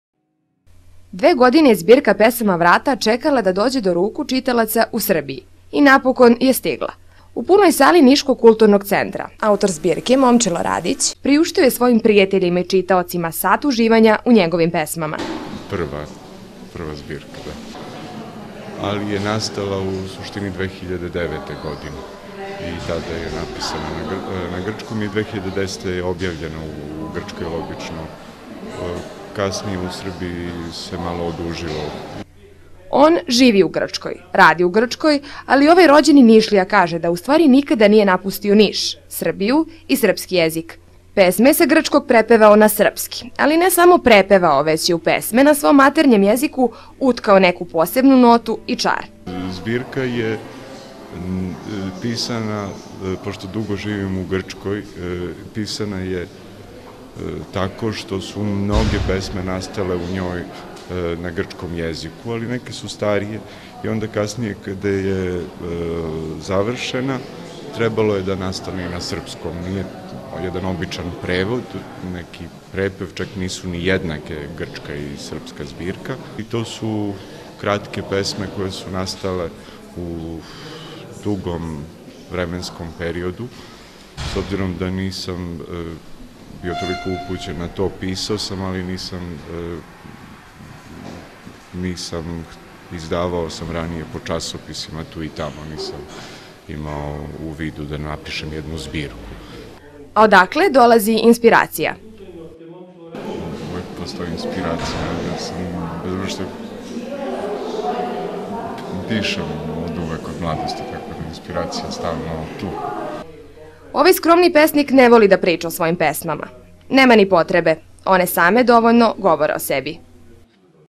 Momčilo Radić "VRATA" - promocija knjige poezije u Nišu
Priča Tamare Milojević